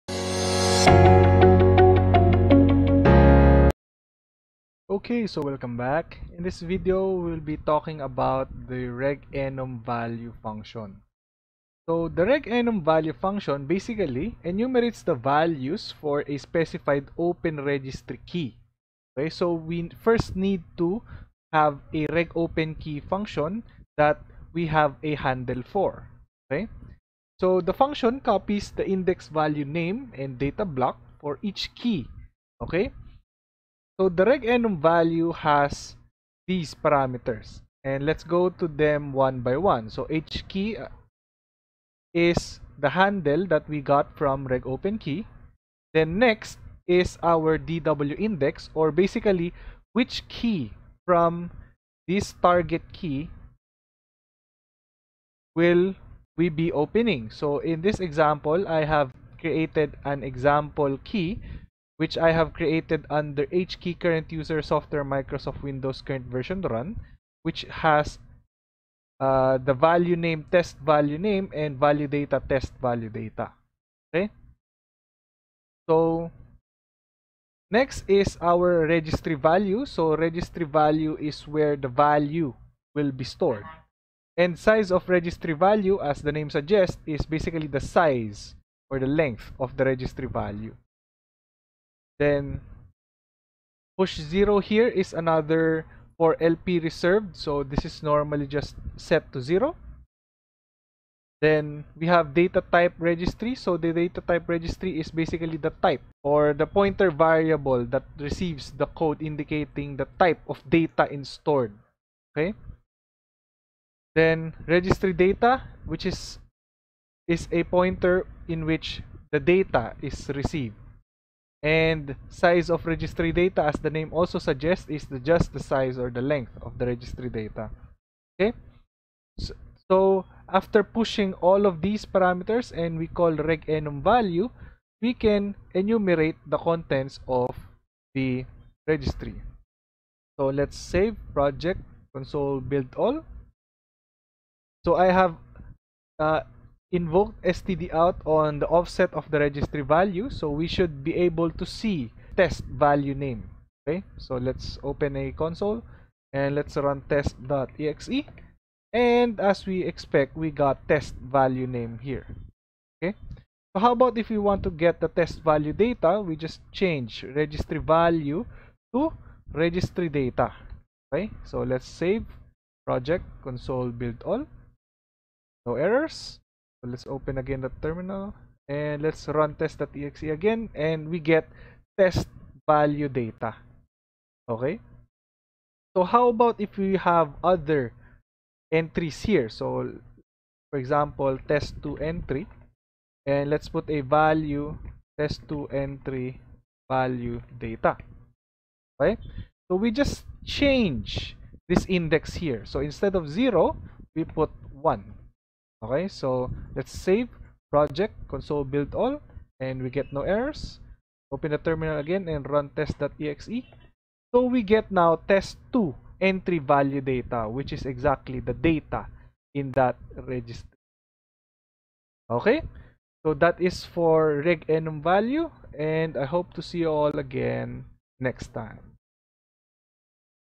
How to Enumerate Registry Values - RegEnumValue | MASM32 Assembly Programming
In this video, you will be learning How to Enumerate Registry Values using the RegEnumValue function